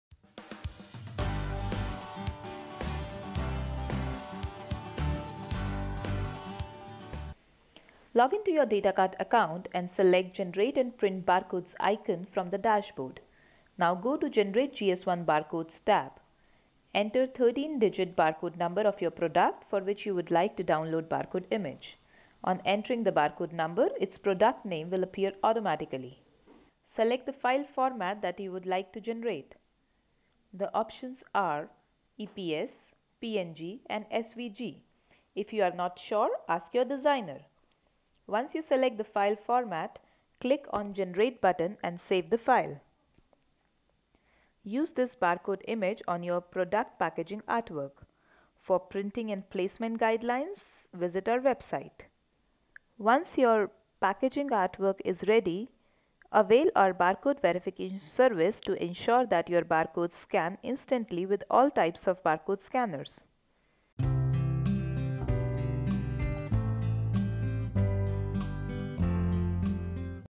How to download EAN-13 barcode image using DataKart?
This video explains the process of downloading barcode image for your product packaging.